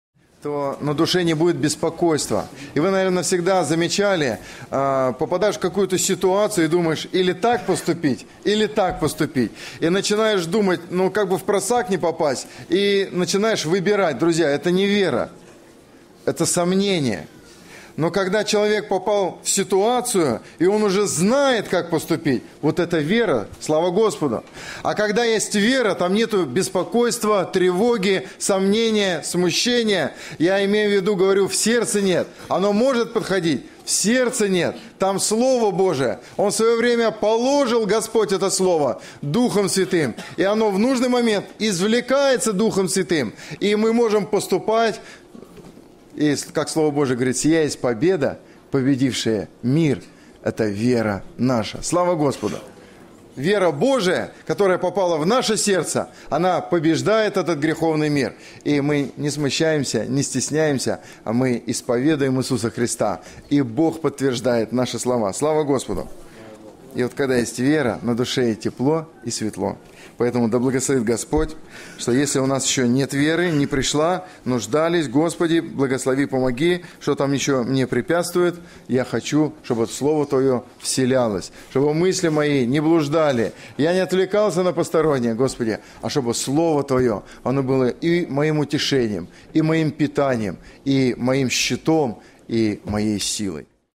Белый Олег - О вере (Проповедь)
Богослужение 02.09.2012. Белый Олег - О вере (Проповедь)